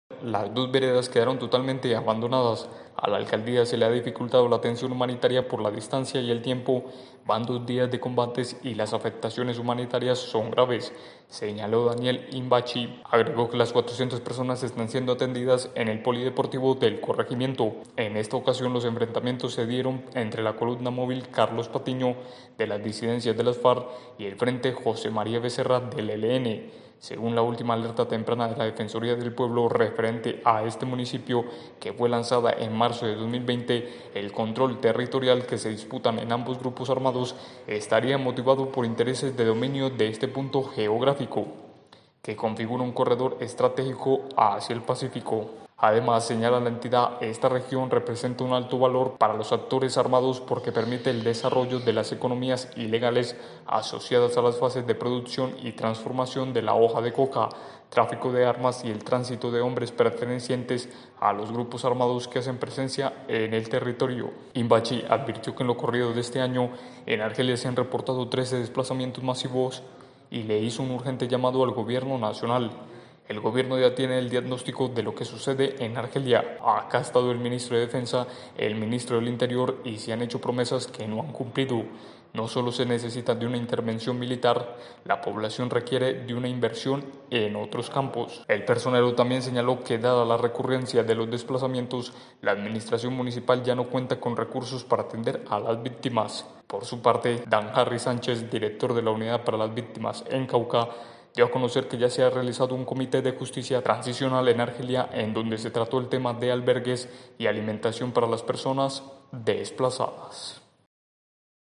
400 PERSONAS SE DESPLAZARON EN ARGELIA, CAUCA, POR ENFRENTAMIENTOS
Un nuevo desplazamiento se reportó en las últimas horas desde el municipio de Argelia, en Cauca. Desde el lunes 6 de septiembre, 400 personas de 80 familias salieron de las veredas El Pinche y Las Vegas luego de que se presentaran enfrentamientos entre dos facciones de los grupos armados ilegales que hacen presencia en la zona.